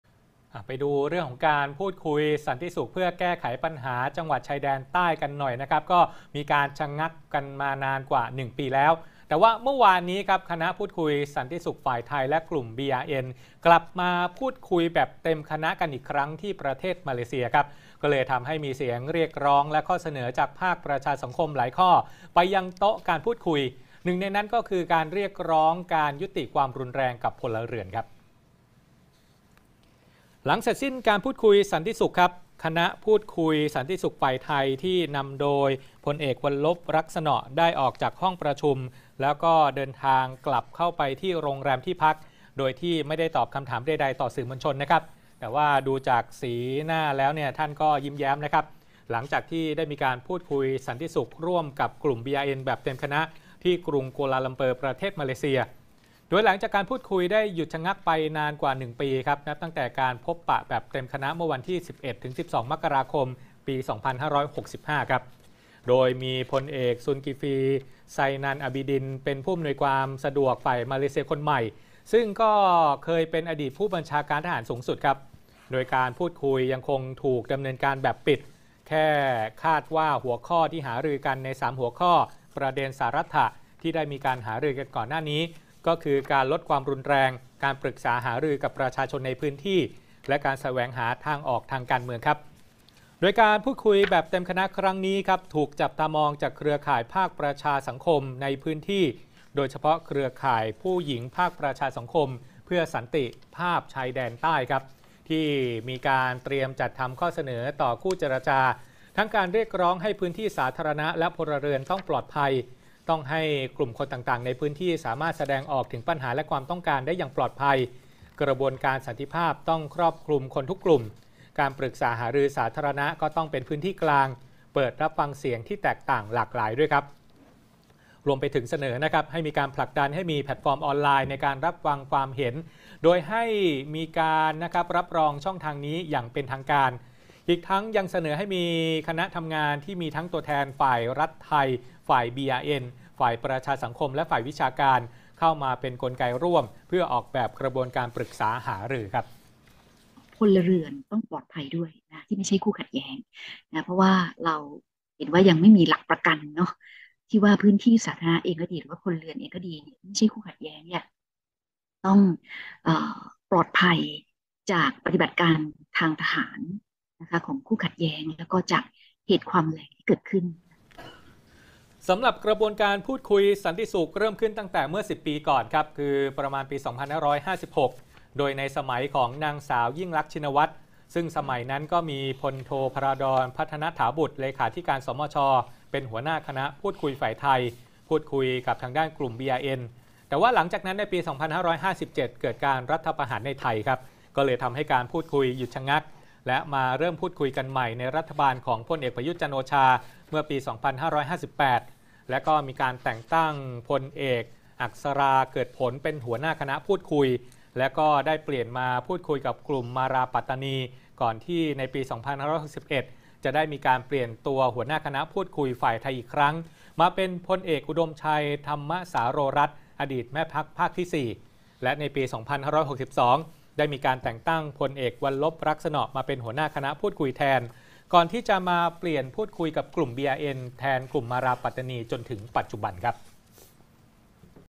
พูดคุยสันติสุขหลังชะงักมานานกว่า 1 ปี
หลังการพูดคุยสันติสุขเพื่อแก้ปัญหาจังหวัดชายแดนภาคใต้หยุดชะงักไปกว่า 1 ปี เมื่อวานนี้ คณะพูดคุยสันติสุขฝ่ายไทย และกลุ่มบีอาร์เอ็น กลับมาพูดคุยแบบเต็มคณะอีกครั้งที่ประเทศมาเลเซีย ทำให้มีเสียงเรียกร้องและข้อเสนอจากภาคประชาสังคมหลายข้อไปยังโต๊ะการพูดคุย หนึ่งในนั้นคือ การเรียกร้องการยุติความรุนแรงกับพลเรือน